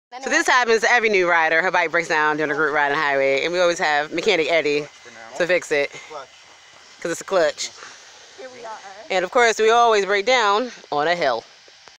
2019 NJLMR sportsbike ride to PA _ Resa honda cbr300 had clutch issue
2019 NJLMR sportsbike ride to PA _ Resa honda cbr300 had clutch issue
So Ali and Eddie was trying to trouble shoot and figure out what is wrong with it. Ahh never boring with njlmr